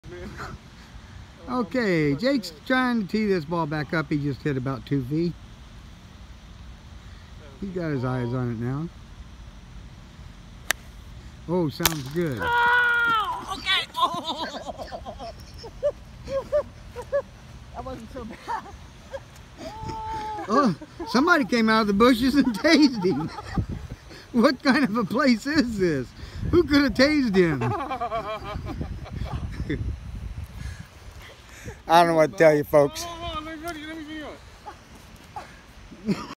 #3 Tazer golf with the Parrish Family Palm Springs vacation
My family can get a little wild. My father gives the play by play while my brother takes one for the team.